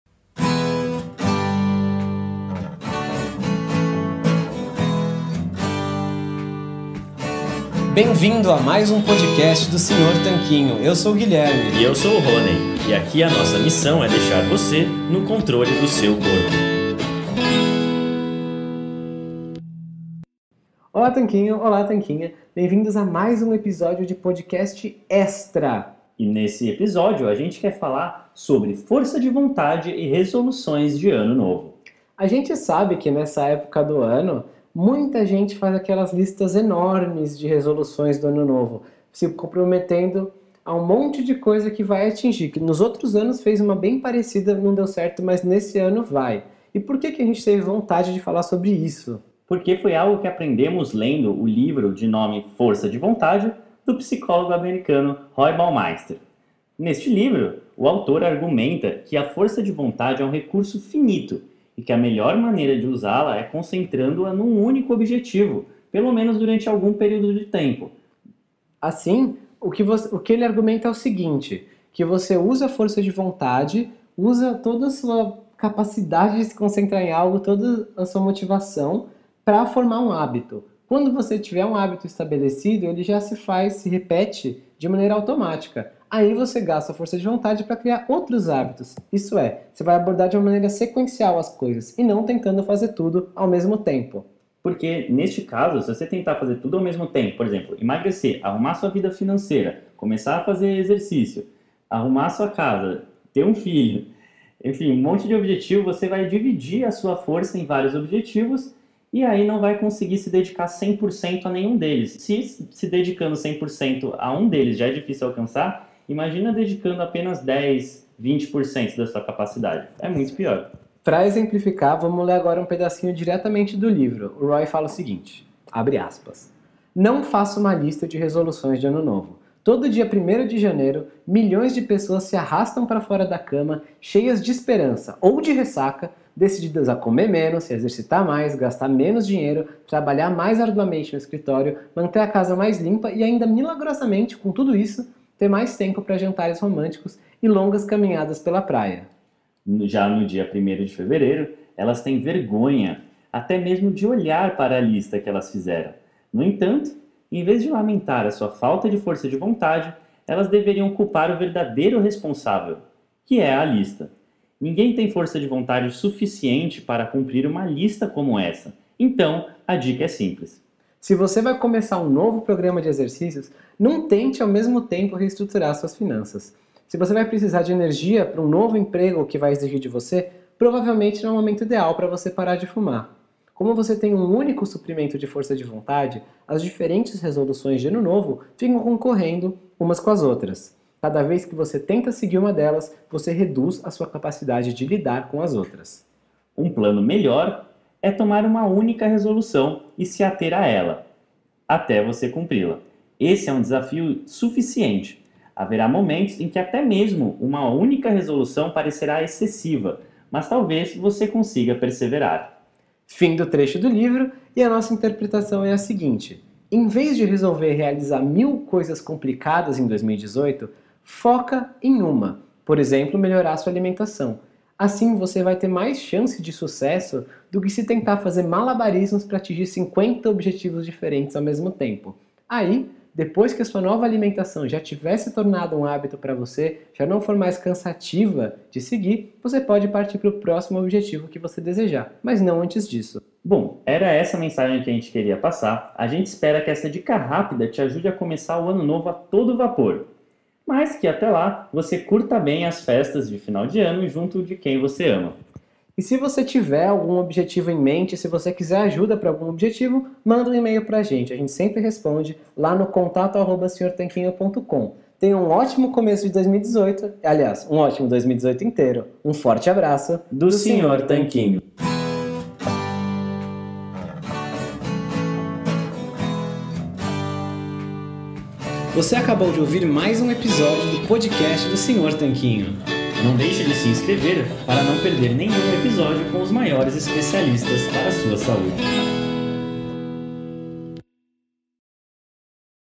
Podcast Extra #009 - Como Ter Sucesso Em 2020 (Jogue Sua Lista De Resoluções No LIXO)
Por Que Você Deveria Jogar Sua Lista De Resoluções De Ano Novo No Lixo

Neste episódio, vamos te contar o que aprendemos lendo um livro Best-Seller do psicólogo americano Roy Baumeister.

Com base na psicologia, vamos te ensinar como garantir seu sucesso em 2020 - simplesmente por jogar sua lista de resoluções na lata do lixo.

Então hoje vamos te ensinar uma abordagem muito mais inteligente do que a de criar esse tipo de listas.

E que você pode usar para garantir que vai emagrecer - e manter o peso - de 2020 para frente.

OFERECIMENTO

- Compre ingredientes low-carb de alta qualidade com preços competitivos - conheça a loja Tudo Low-Carb: